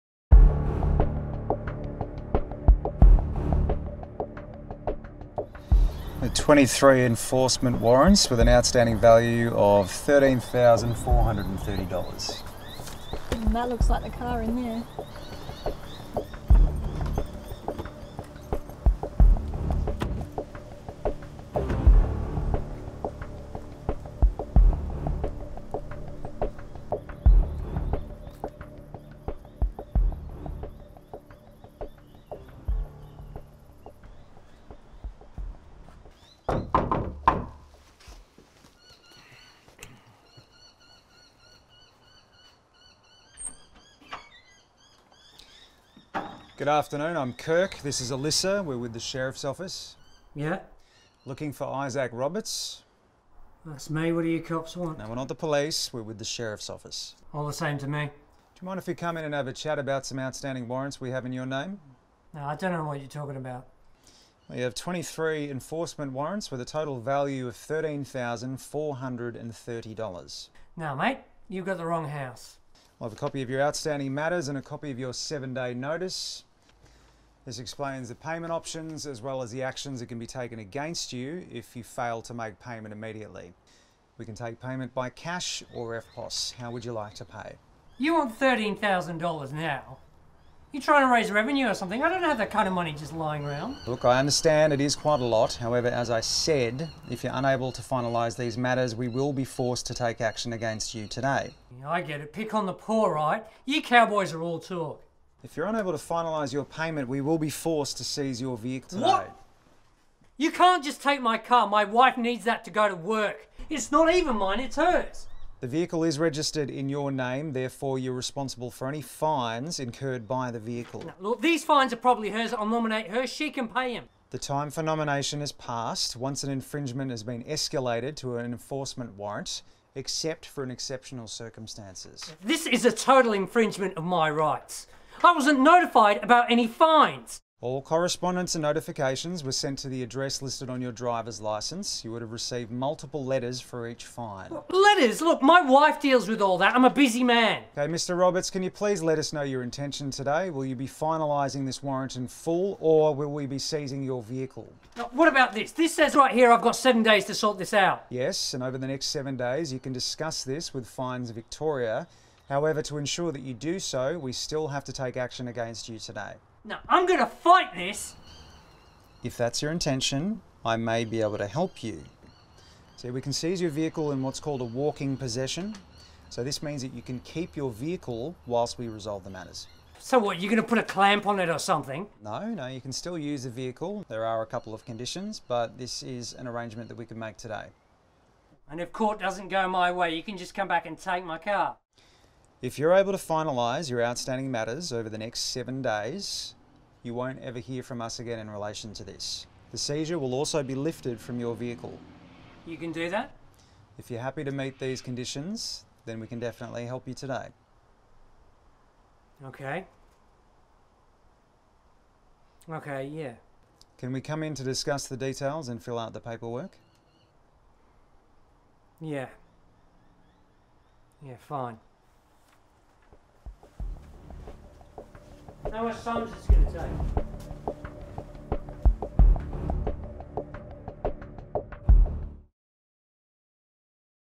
At work with Victoria’s sheriff’s officers serving the community
Follow two of our sheriff’s officers as they execute a warrant and see if you have the communication skills to join our team.